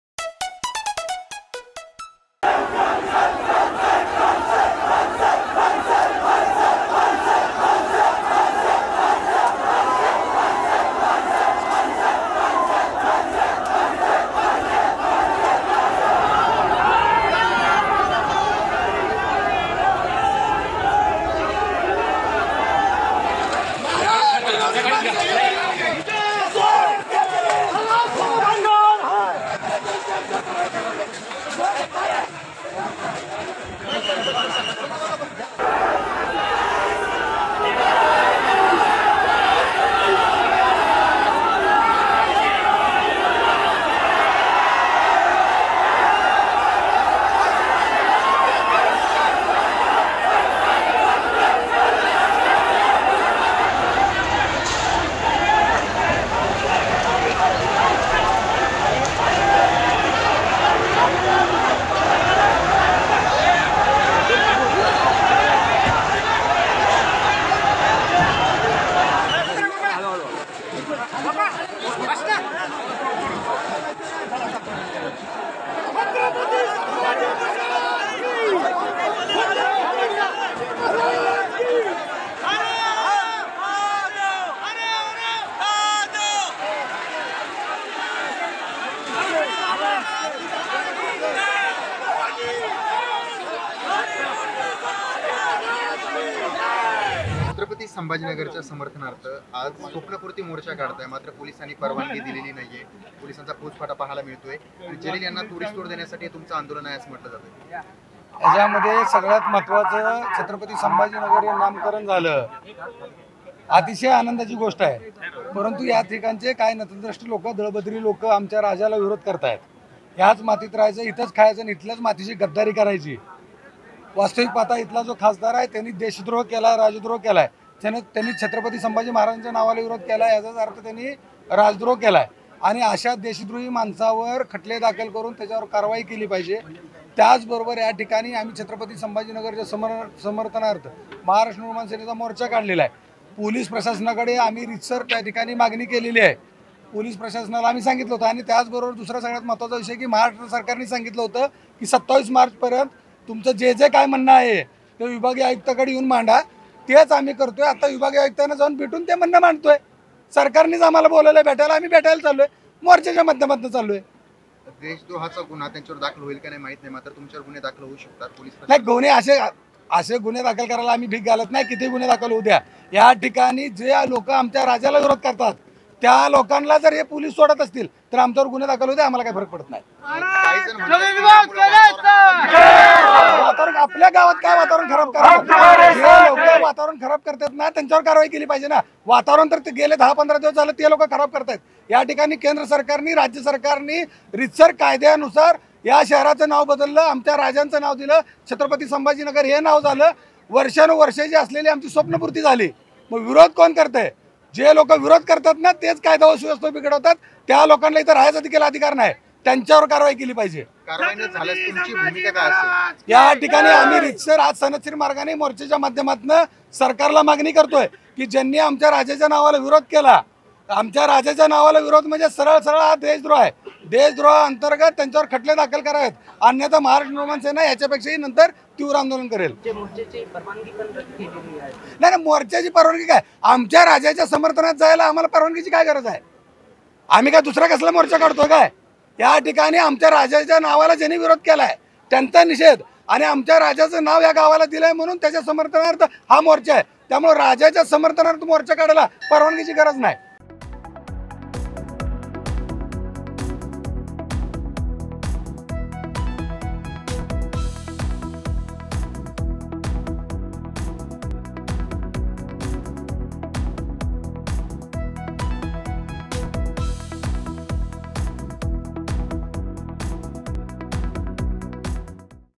MNS Protest Live | मनसेचा Imtiyaz Jaleel यांच्याविरोधात मोर्चा, पोलिसांकडून धरपकड | Sambhajinagar
महाराष्ट्र नवनिर्माण सेनेनं आज इम्तियाज जलील यांच्या विरोधात जोरदार मोर्चा काढला. छत्रपती संभाजीनगरमध्ये आयोजित करण्यात आलेल्या या मोर्चादरम्यान चांगलाच गोंधळ उडाला.